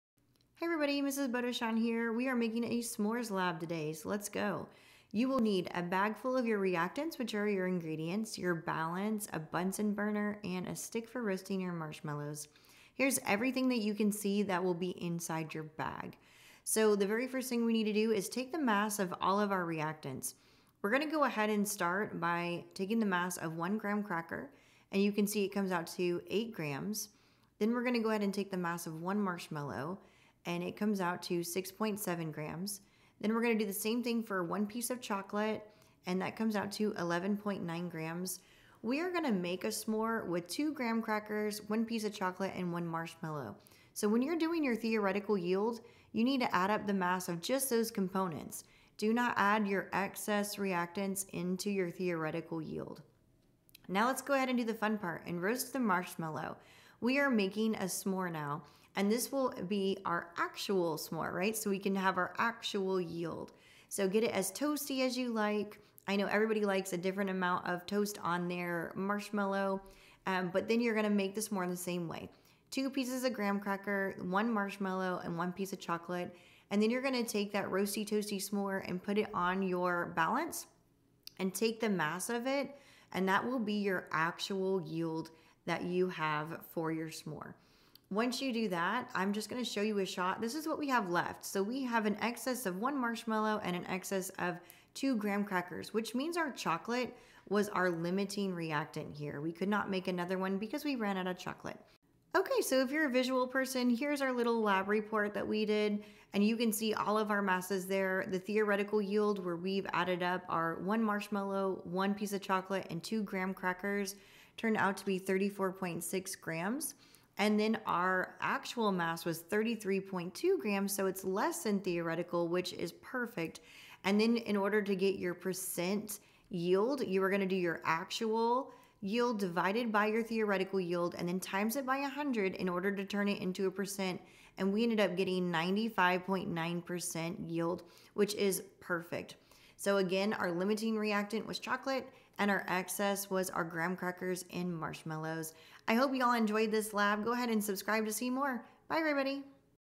S'mores Lab | Chemistry Percent Yield, Limiting Reactant, & Excess Reactant Experiment | S'more Lab
Mrs. Bodechon will walk you through how to conduct the S'mores Lab. She will show you all of the steps and help you calculate the theoretical yield,  actual yield, and the percent yield. Plus this is just a fun and awesome science experiment.